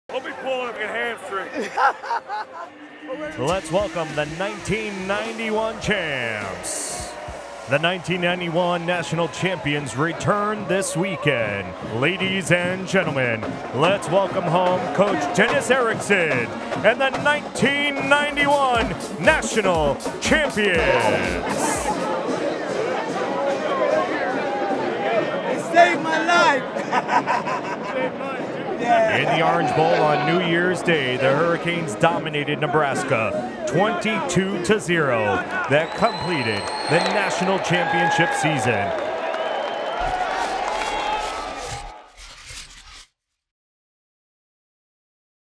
Miami Hurricanes 1991 Champions Reunion
The national champs reunited for a weekend in Miami and went to midfield at halftime of the Miami-Michigan State game